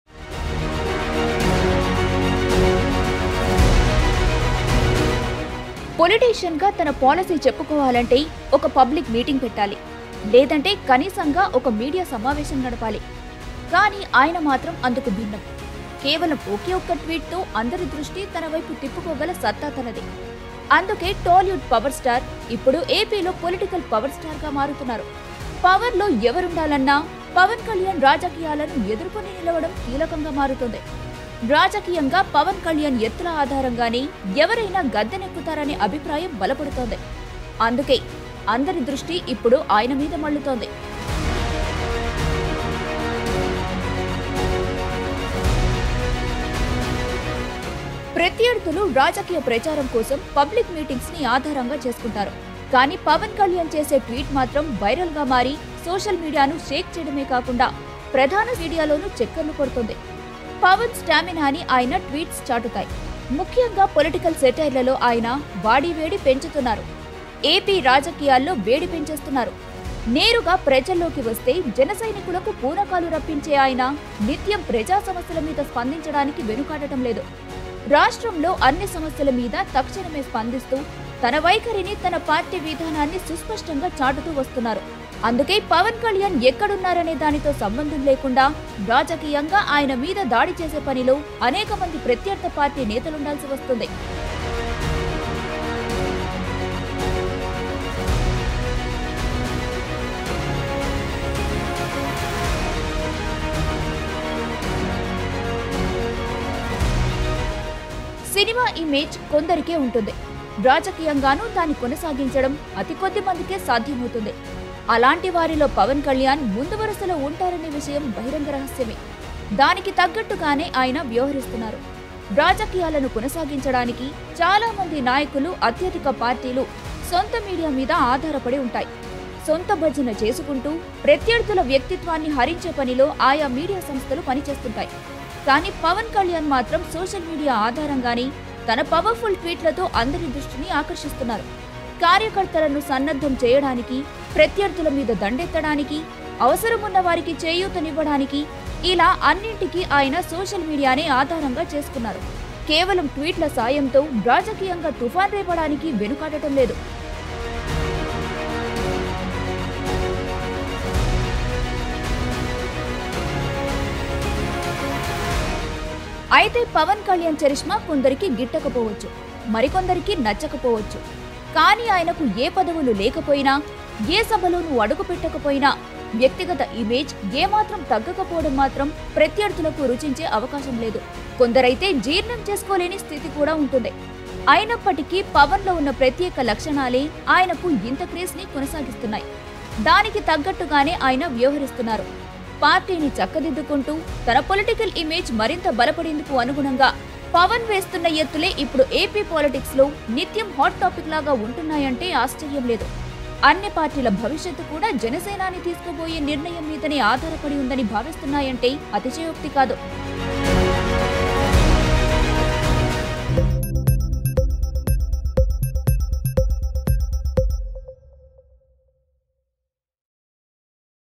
Janasena Chief Pawan Kalyan Gains Huge Attention Through His Powerful Tweets | Mango News
Janasena Chief Leader Pawan Kalyan Gains Huge Attention through his Powerful Tweets.